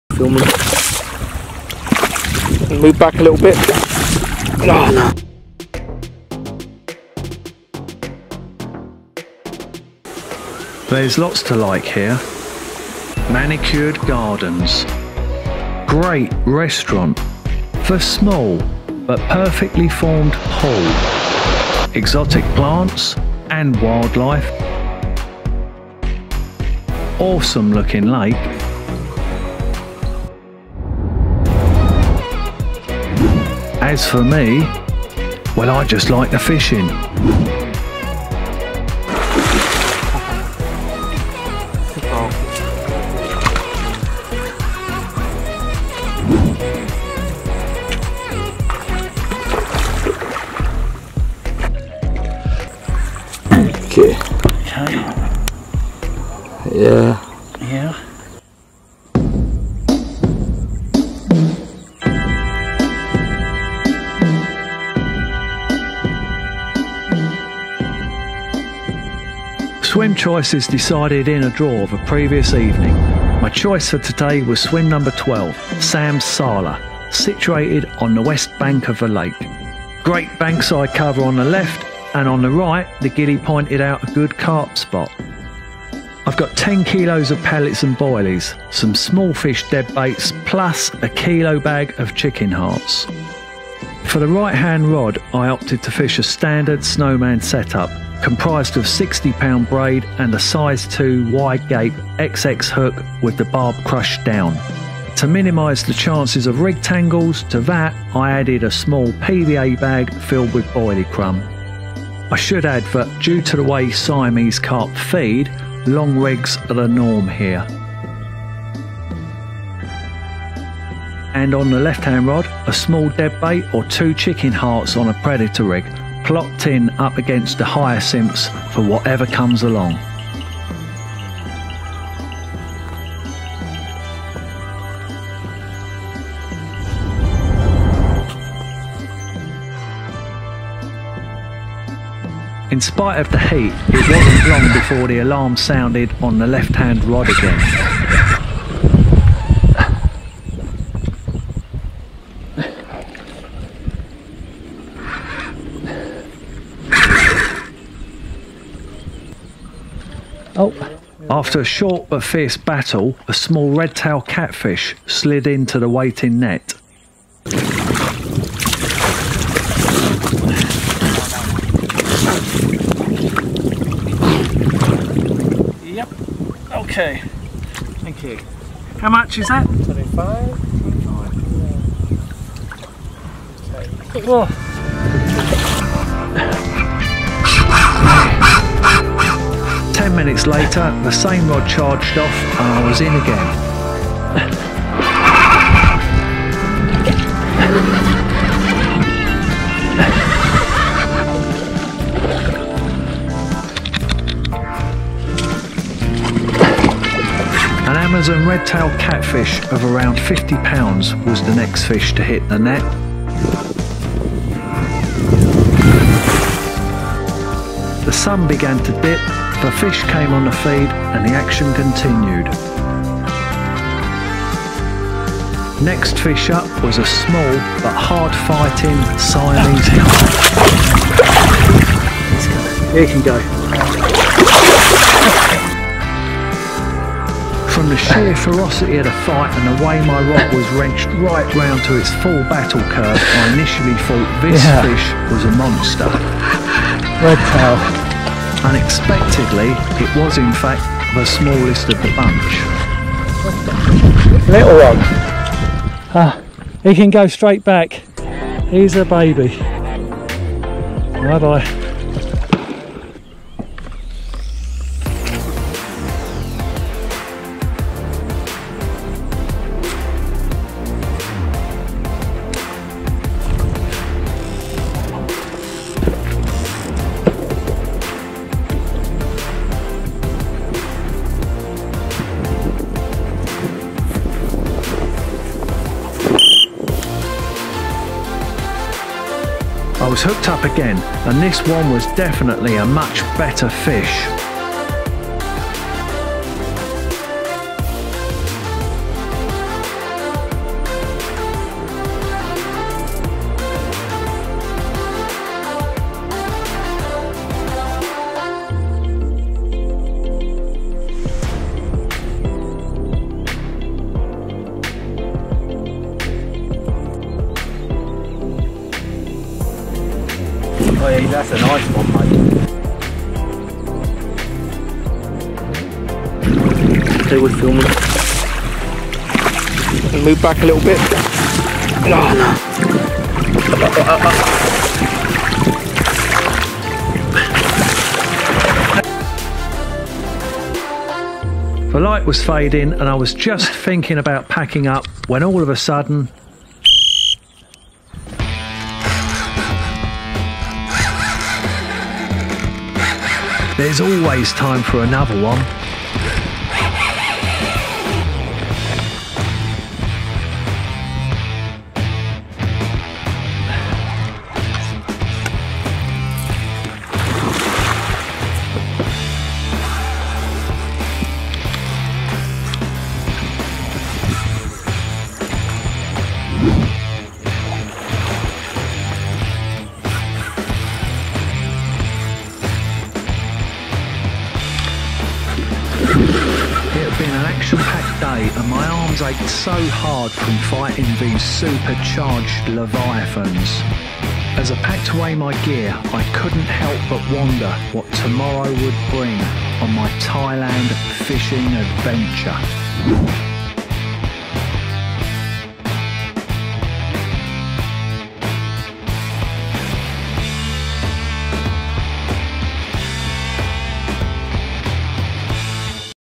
Fishing in Thailand at Jurassic Mountain Fishing Resort - Part 2
Here There be Monsters!

Welcome to part 2 of my Thailand Fishing
Adventure at Jurassic Mountain Fishing Resort.

This place is awsome... and home to real life River Monsters.
The fish that live here will absolutely pull your string and test not only your gear but your skill as an angler to the utmost!

Strap yourself in and get ready for arm aching battles with plenty of POV camera action as I hook up to some real super-charged leviathans!

Filmed on location at Jurassic Mountain Resort
 and Fishing Park Thailand. Feb 2020.